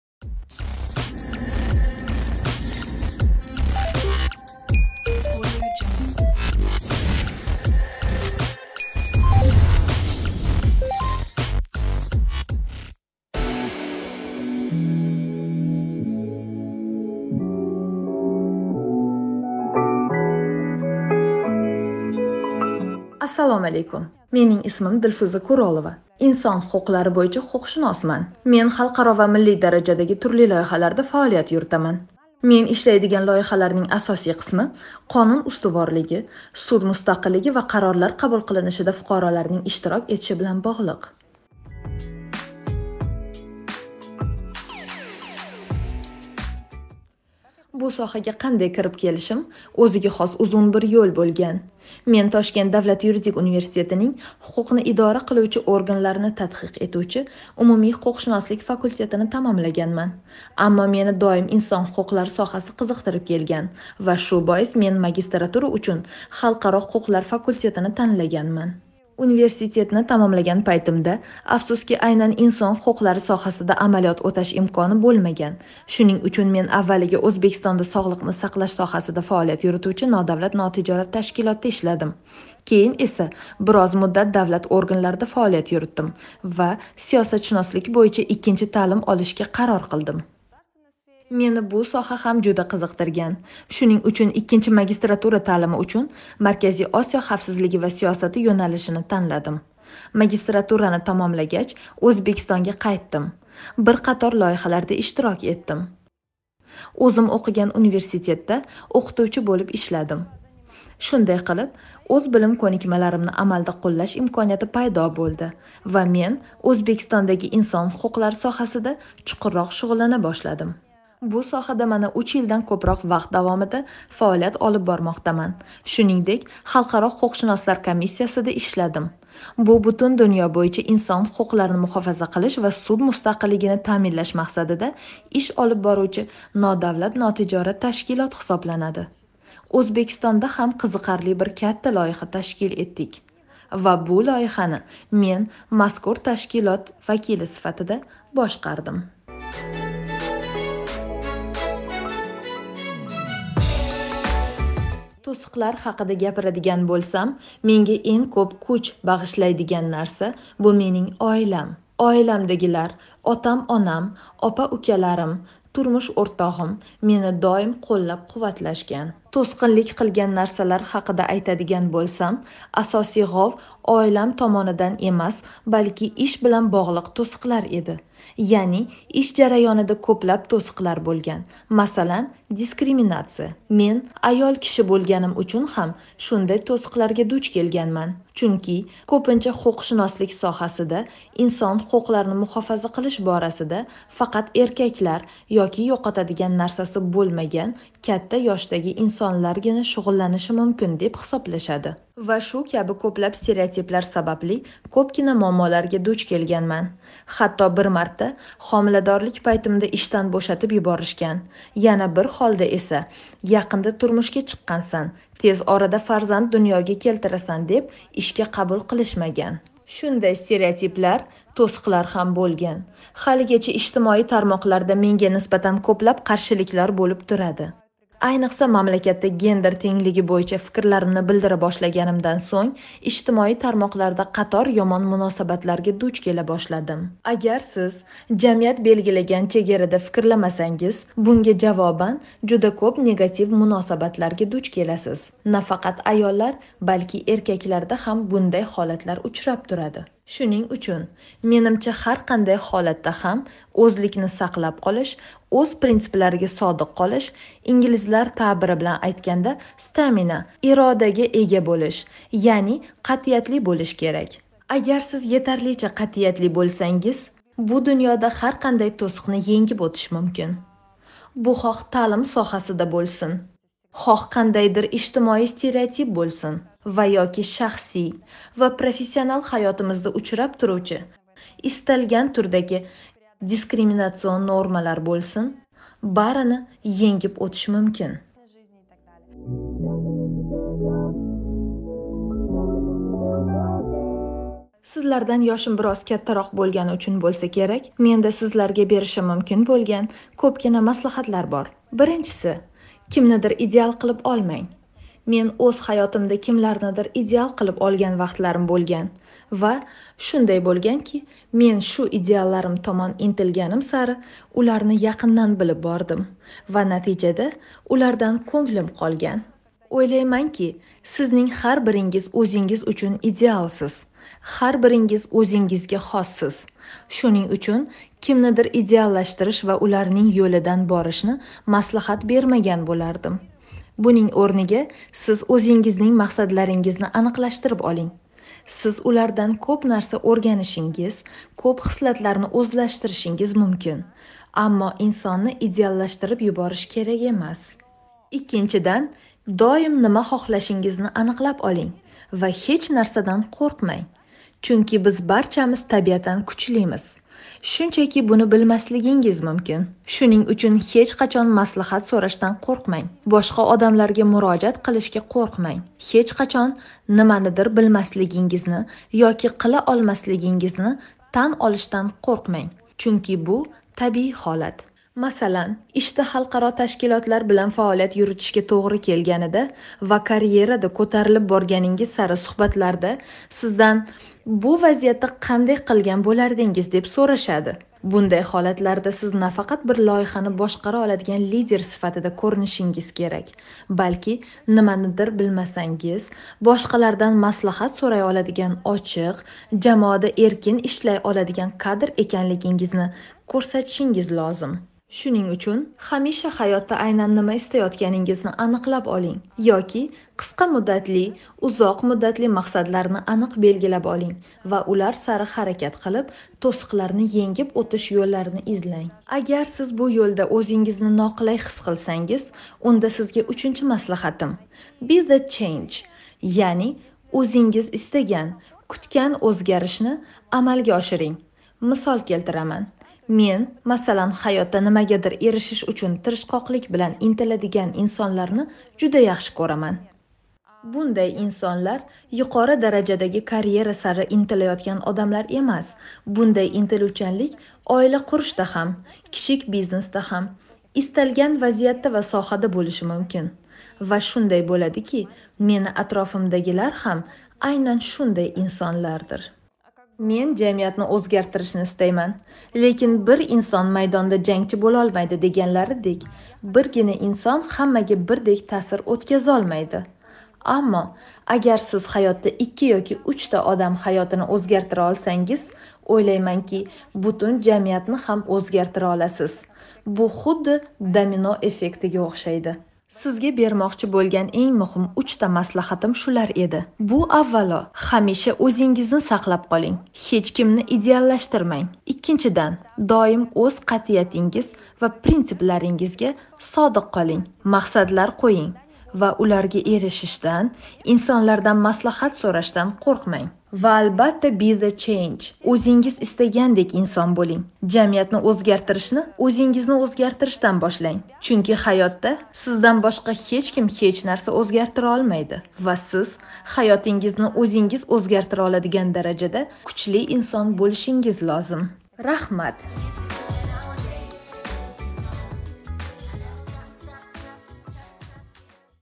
"Maqsadlaringizni belgilab oling va ularga erishishdan qoʻrqmang!"
"Girls for Girls" loyihasining navbatdagi mehmoni huquqshunos Dilfuza Kurolova.